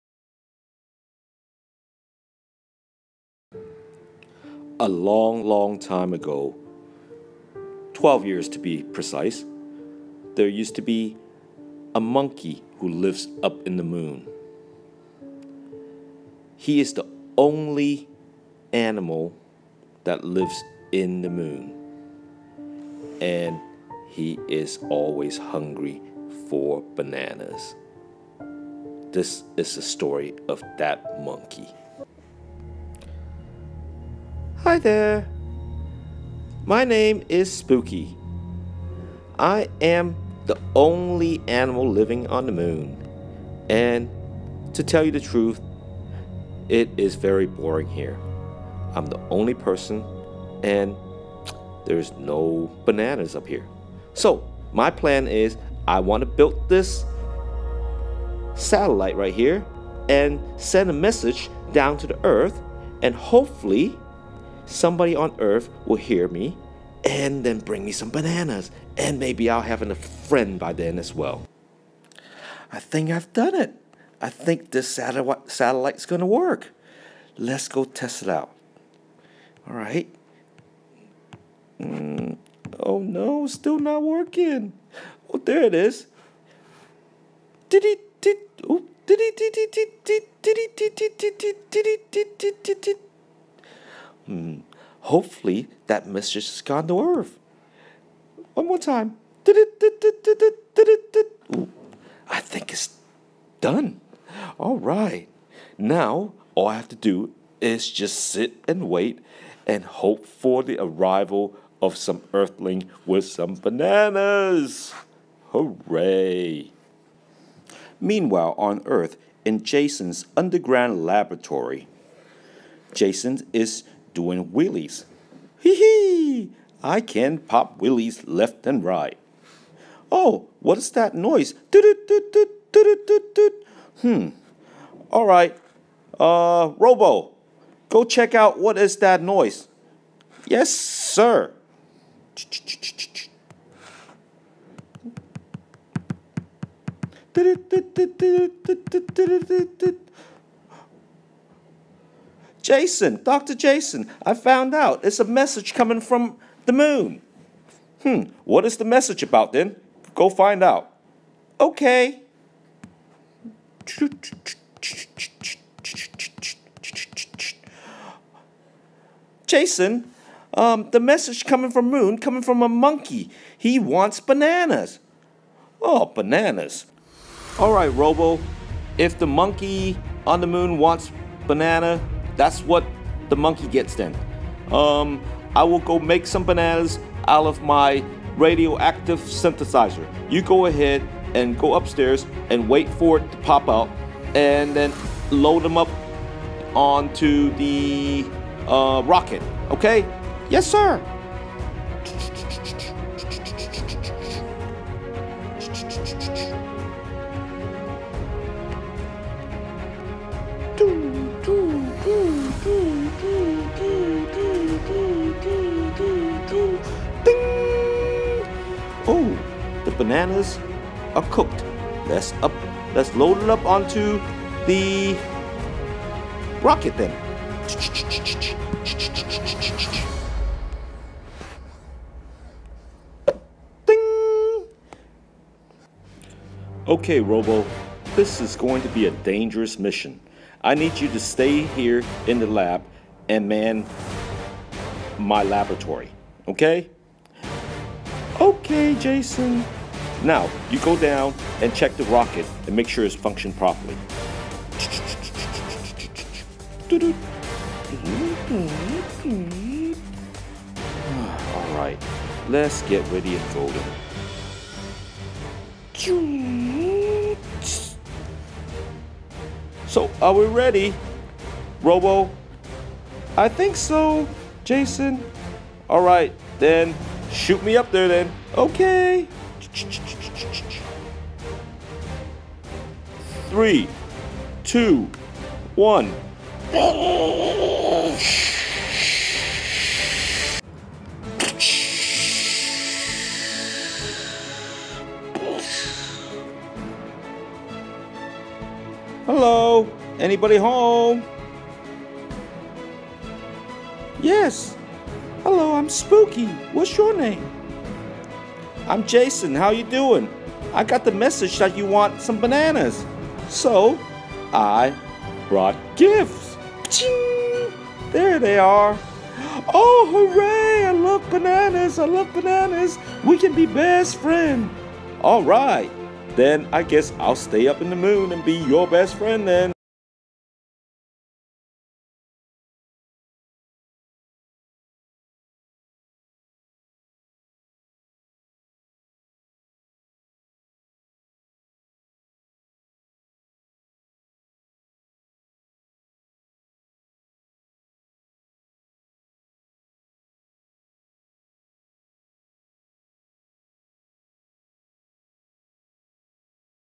Toontastic Demo
This is a short demo that I'd made on the go. Not the best quality. Toontastic is a Google Product for iPad which allows young children to create their own cartoon. The best feature is that it is in a story mountain format where children can learn the basic features of a story--beginning, build-up, dilemma, resolution, ending. This is a great companion tool for young children in their story writings.